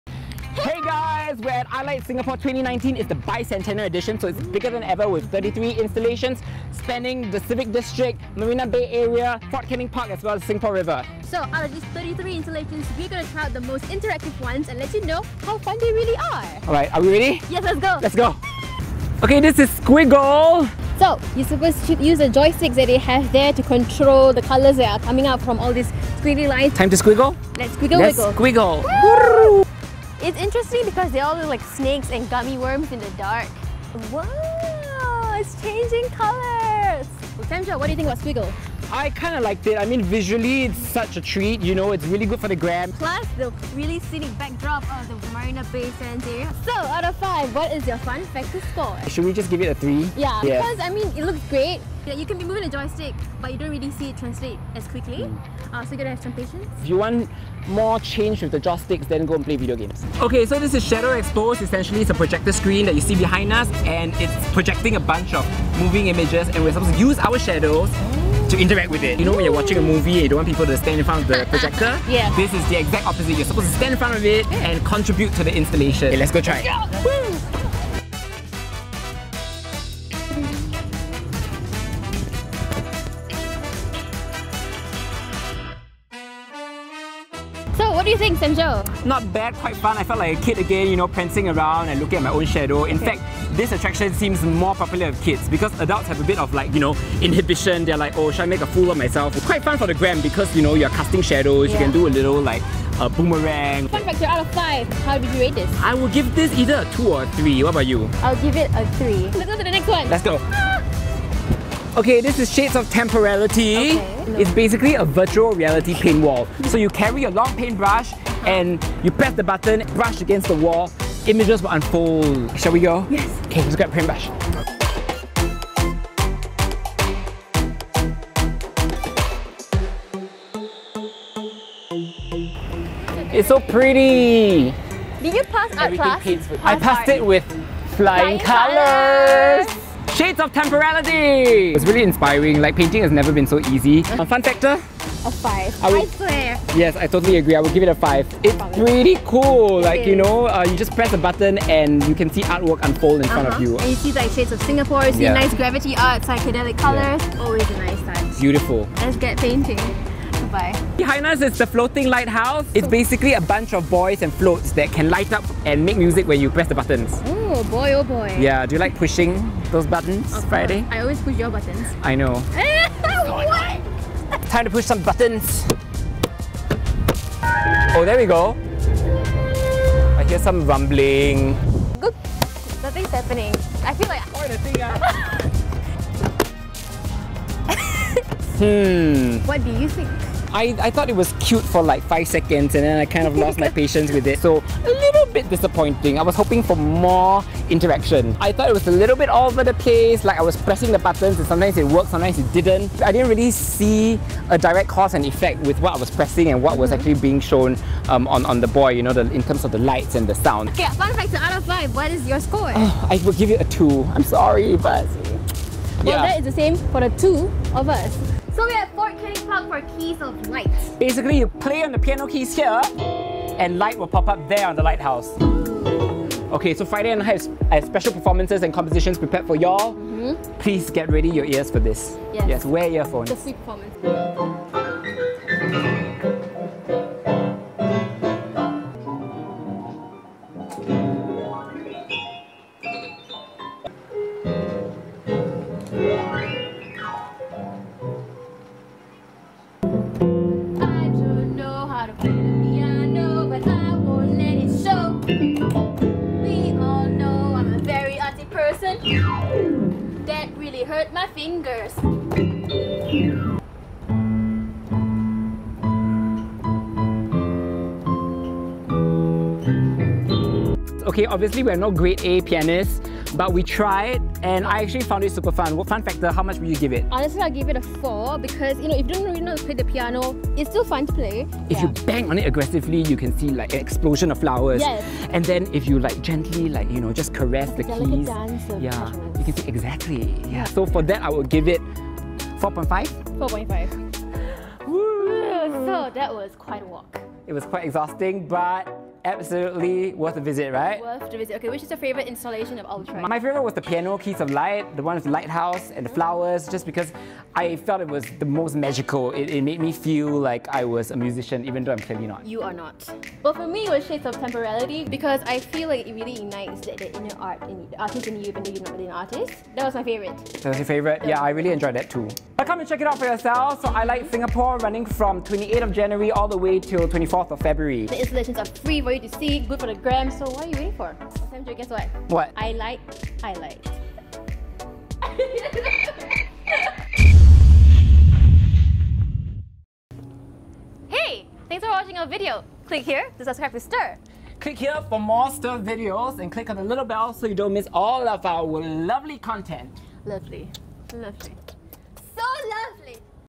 What's LIT at i Light Singapore 2019 | Stirr SG
We unleash our inner artist upon the most interactive art pieces at the Bicentennial edition of i Light Singapore.